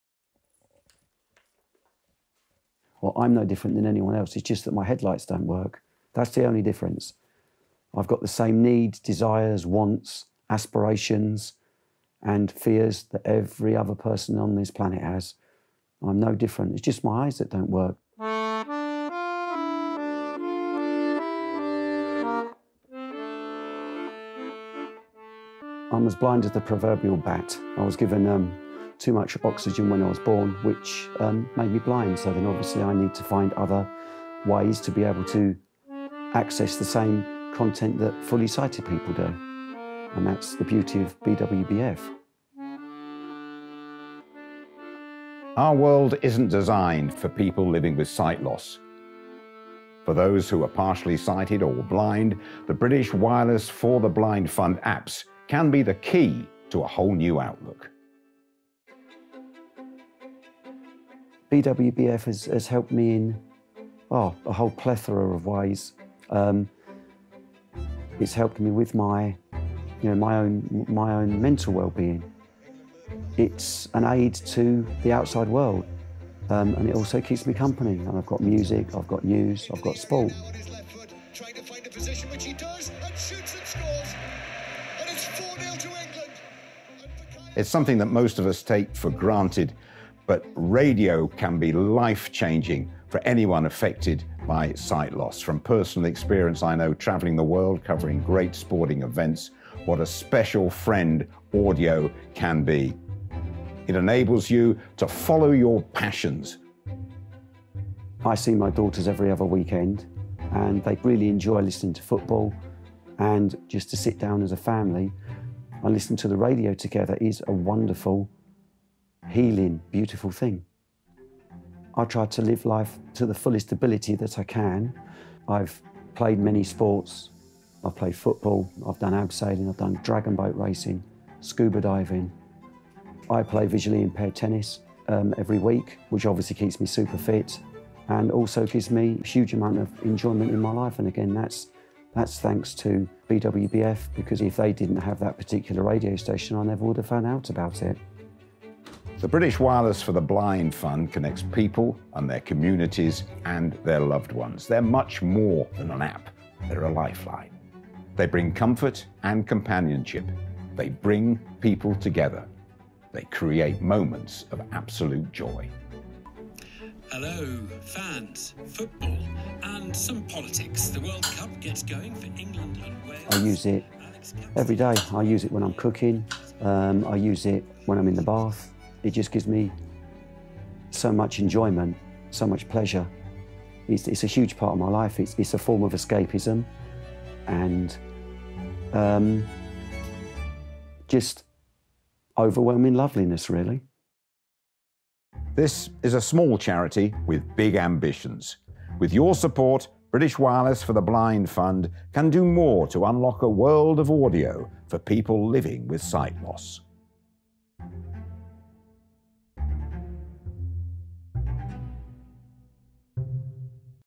Unlocking a world of audio
At British Wireless for the Blind Fund, we believe access to audio can be life changing for someone living with sight loss. Our easy-to-use apps and skills unlock a world of audio content on the internet for people who might otherwise struggle to access it. Watch as Jason, who uses our apps and our patron, Jim Rosenthal, talk about the life-changing difference BWBF makes.

Discover how we can make a difference by visiting our website and following us on social media.